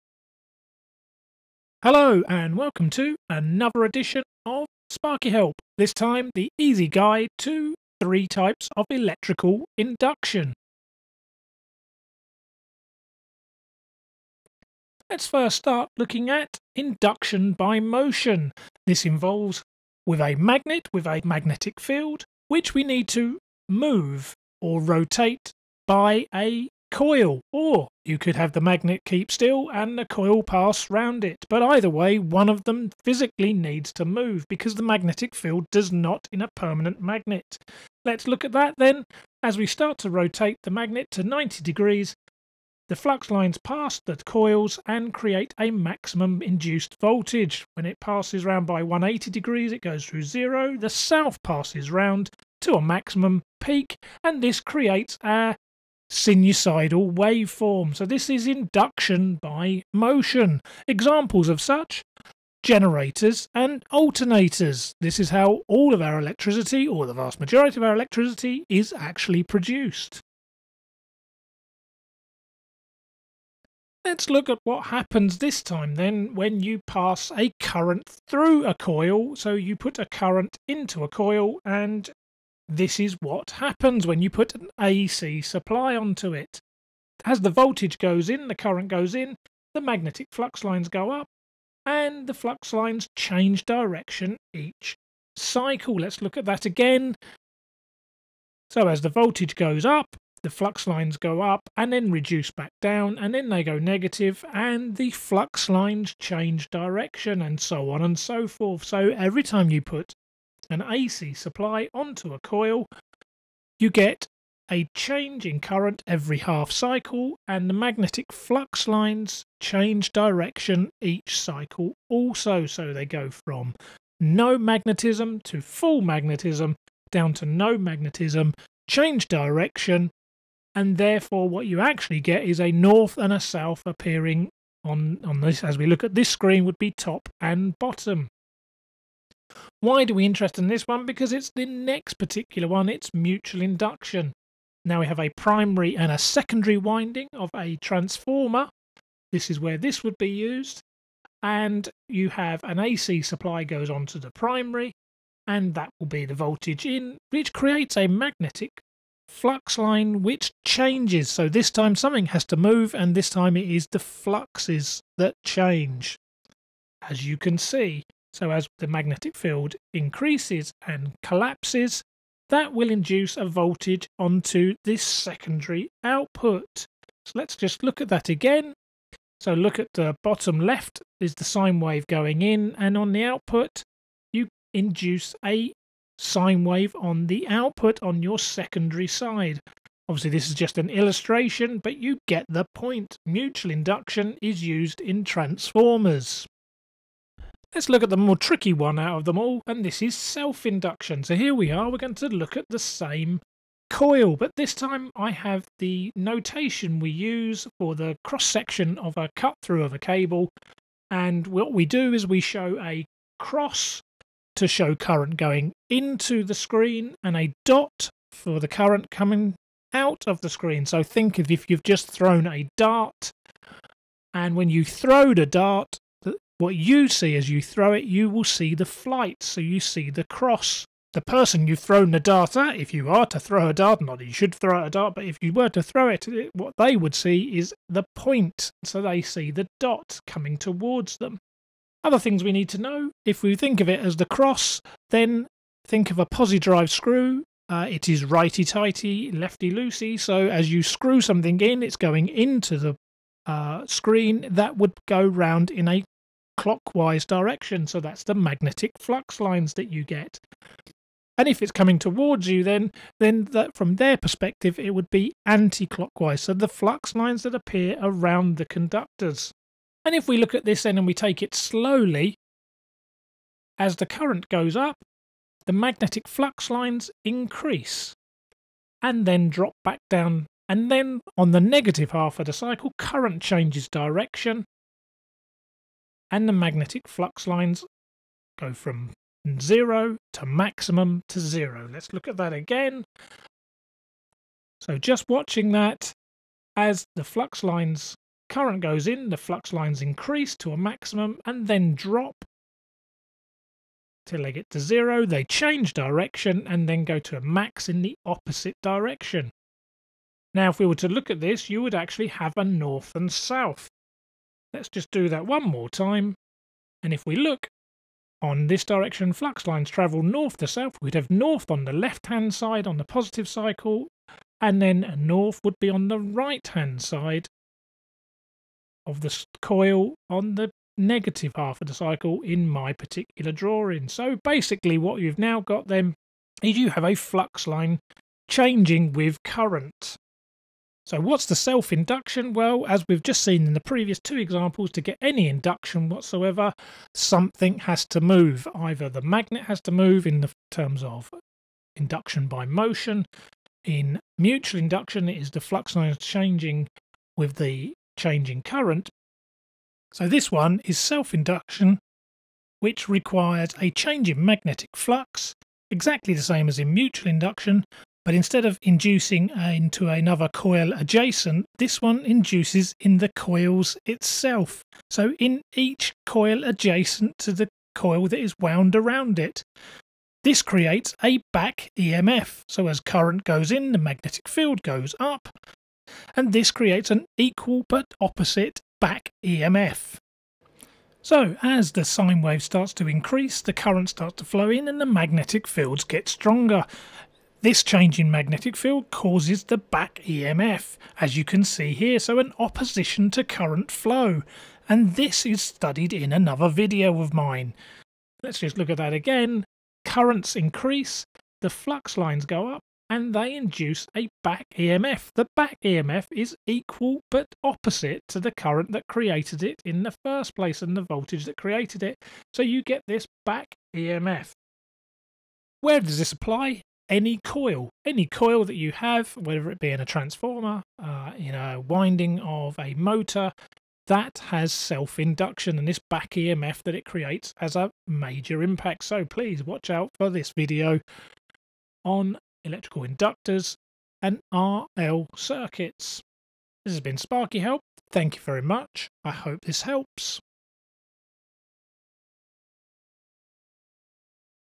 Easy Guide to Three Types of Electrical Induction
Easy Guide to Three types of Electrical Induction, these being Induction by Motion, Mutual Induction & Self Induction. A quick demonstration and explanation of these three types for your help in understanding of electrical principles. Great for those on a level 2 or 3 Electrical course or for any other person studying electrical principles at any other level.  View this video in conjunction with other videos on my YouTube site.  What is a back EMF how does a transformer work how do you generate AC electricity.